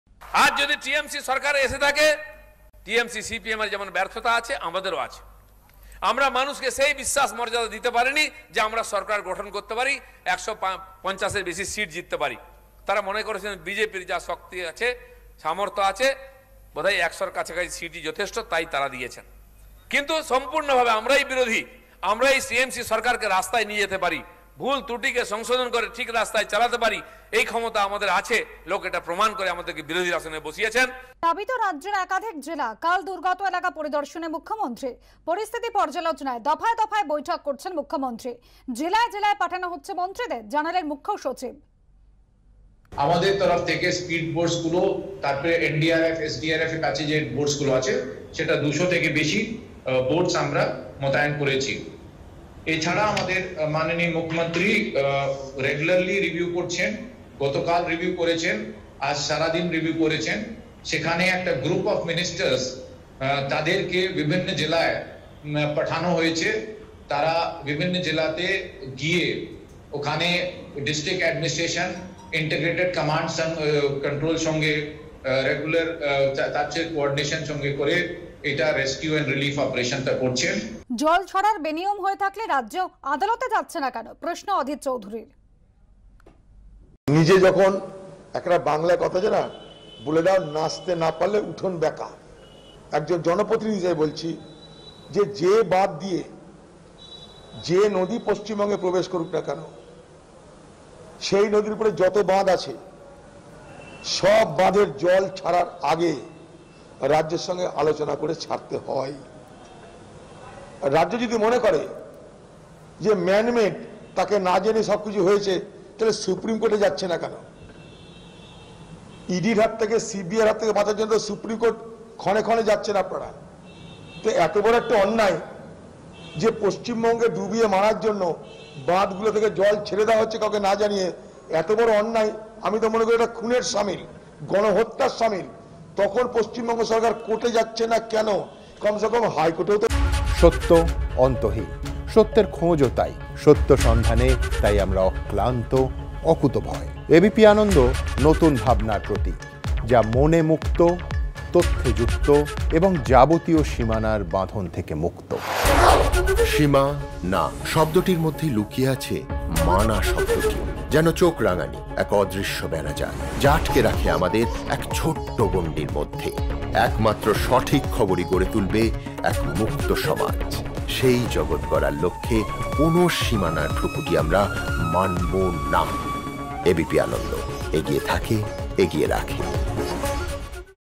Dilip Ghosh: 'সরকার গড়ার মতো বিশ্বাস-মর্যাদা মানুষকে দিতে পারিনি', আত্মসমালোচনায় দিলীপ ঘোষ
'তৃণমূল-সিপিএমের ব্যর্থতা থাকলে, বিজেপিরও ব্যর্থতা আছে। আমরা মানুষকে সেই বিশ্বাস-মর্যাদা দিতে পারিনি, যে আমরা সরকার গড়তে পারি।' ইজেডসিসিতে এক অনুষ্ঠানে আত্মসমালোচনার সুর বিজেপির সর্বভারতীয় সহ সভাপতি দিলীপ ঘোষের গলায়।